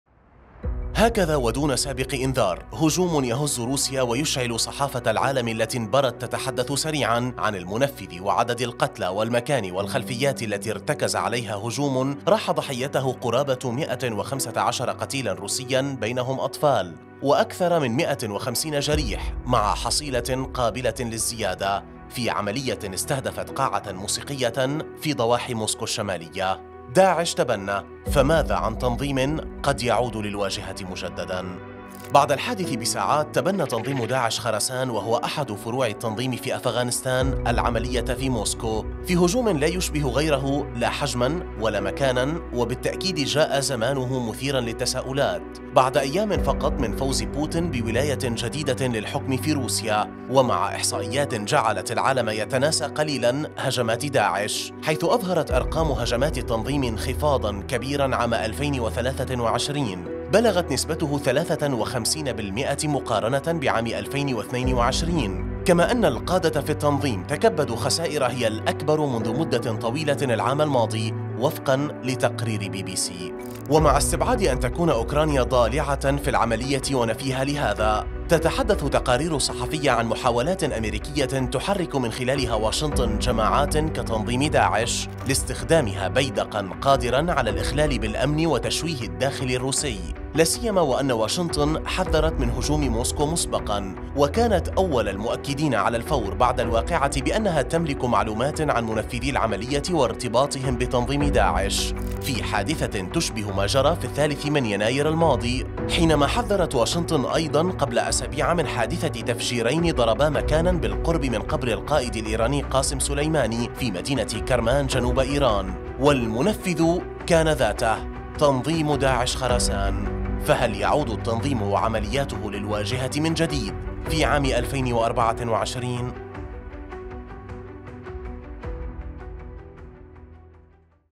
بعد تبنيه الهجوم الدامي بموسكو.. هل يعود "داعـ.ـش" مجددا؟‎
هكذا ودون سابق إنذار، هجوم يهز روسيا ويشعل صحافة العالم التي انبرت تتحدث سريعا عن المنفذ وعدد القتلى والمكان والخلفيات التي ارتكز عليها هجوم راح ضحيته قرابة 115 قتيلا روسيا بينهم أطفال وأكثر من 150 جريح مع حصيلة قابلة للزيادة، في عملية استهدفت قاعة موسيقية في ضواحي موسكو الشمالية، داعش تبنى، فماذا عن تنظيم قد يعود للواجهة مجددا؟